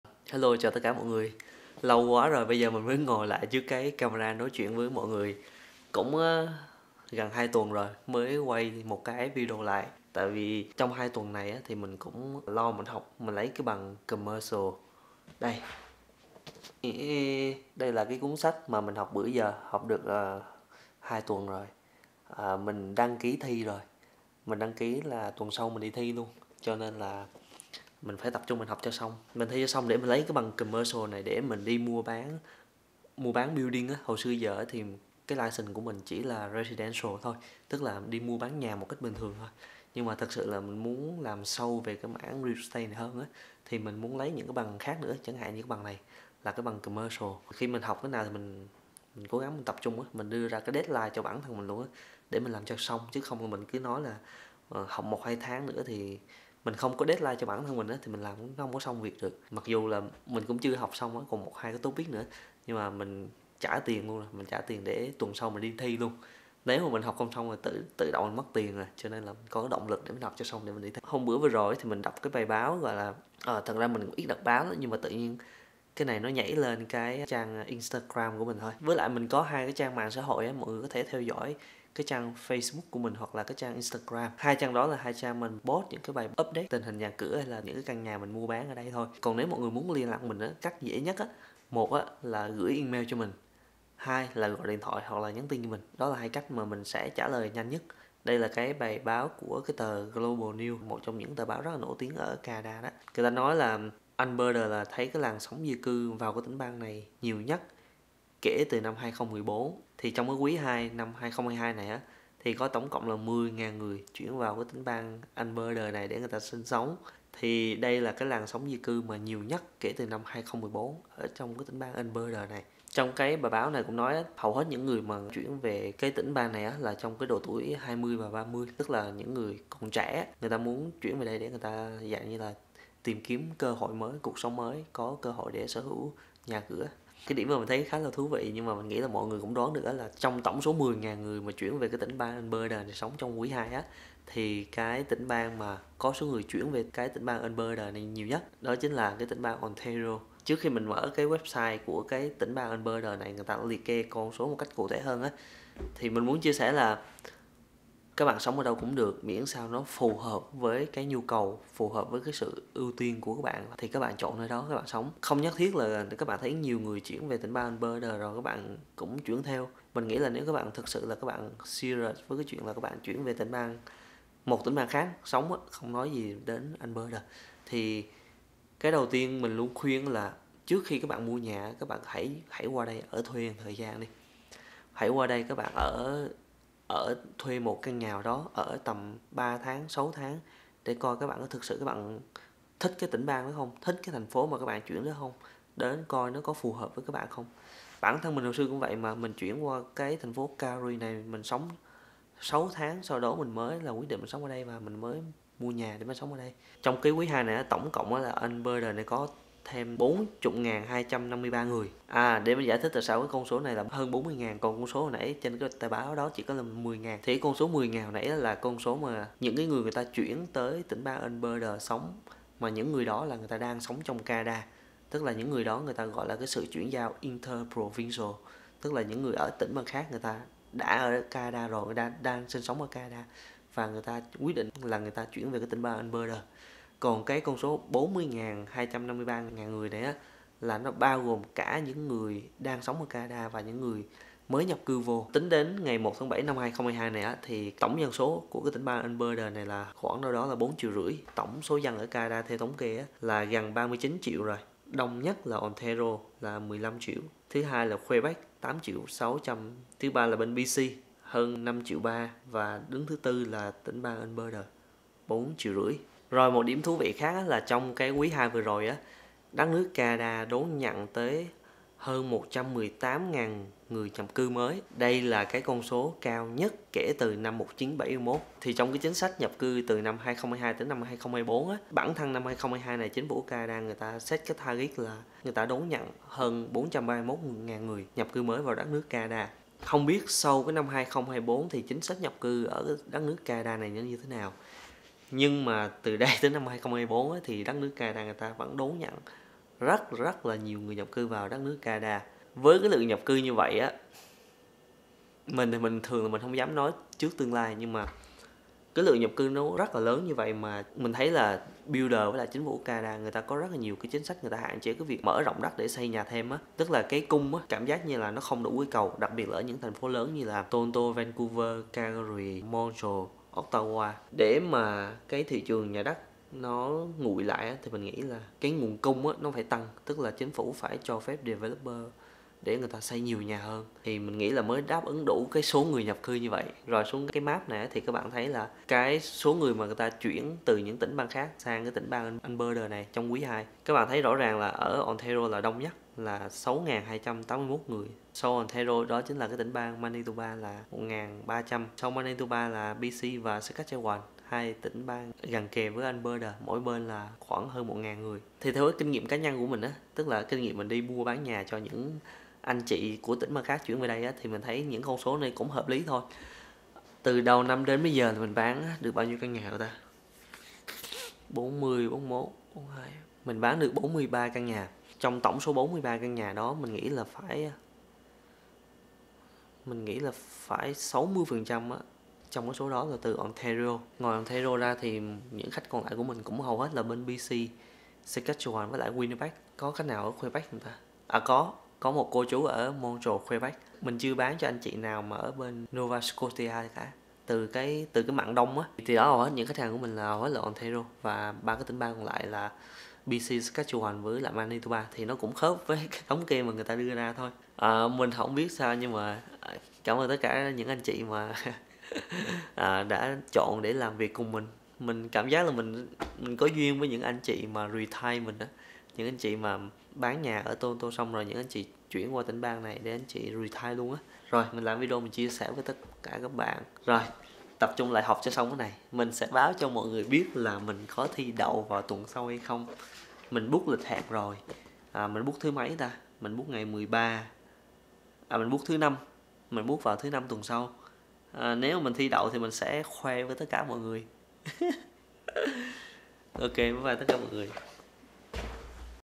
Lượng người Canada chuyển đến Alberta LỚN NHẤT kể từ 2014
Cuộc sống Calgary, tỉnh bang Alberta, Canada.